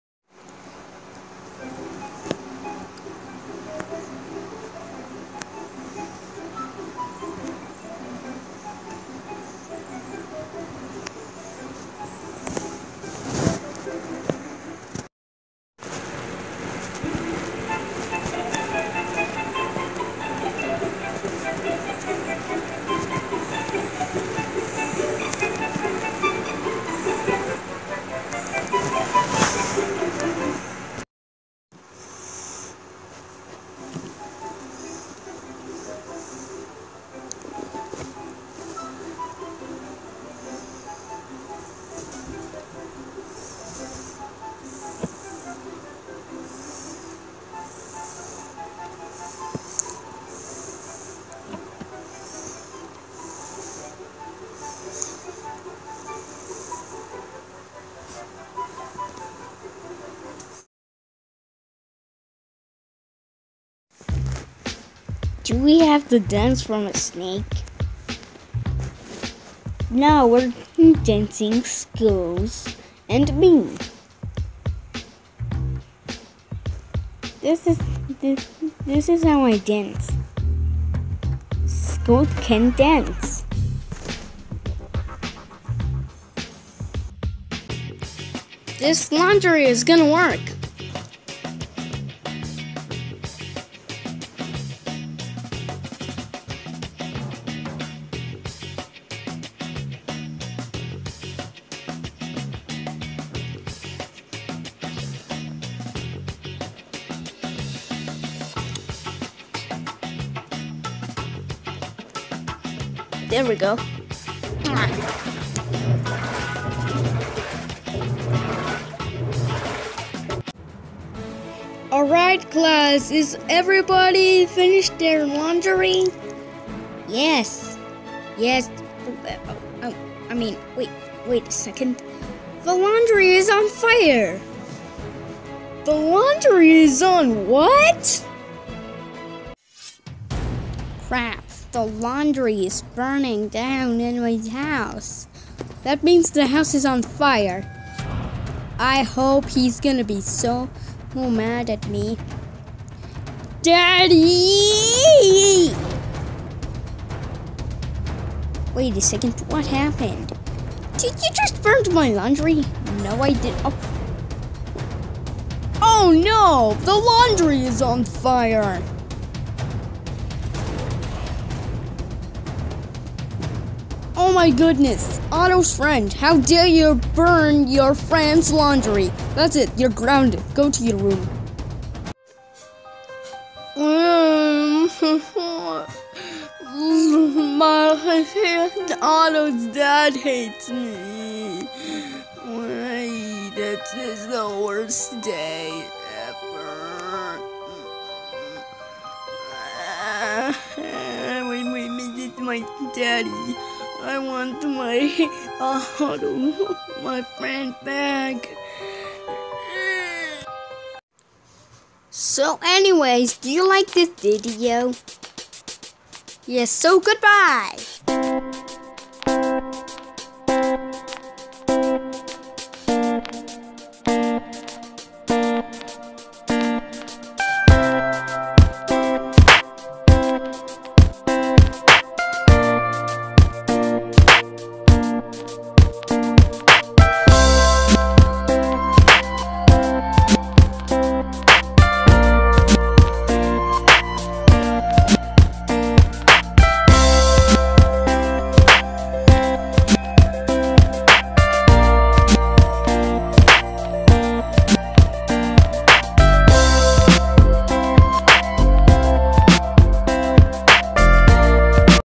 Outto Puts fire in the laundry outto's friend gets grounded
Stupid outto's friend